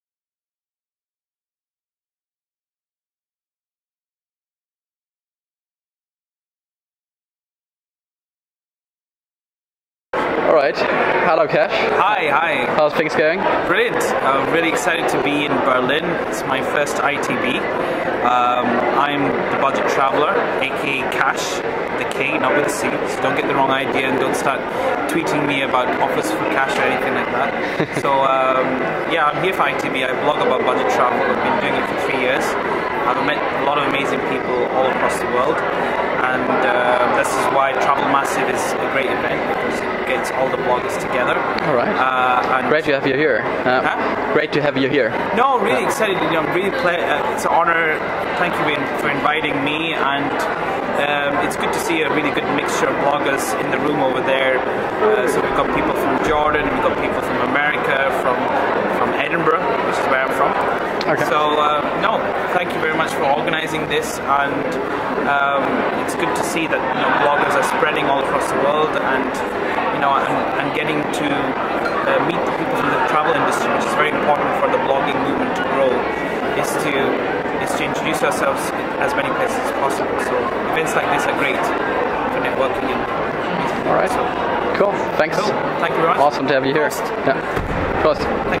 Travel Massive @ ITB Berlin 2012 - Kash (Budget Traveller) - Interview by pocketvillage
Interview with Kash of Budget Traveller at Travelmassive@ITB 2012. by: pocketvillage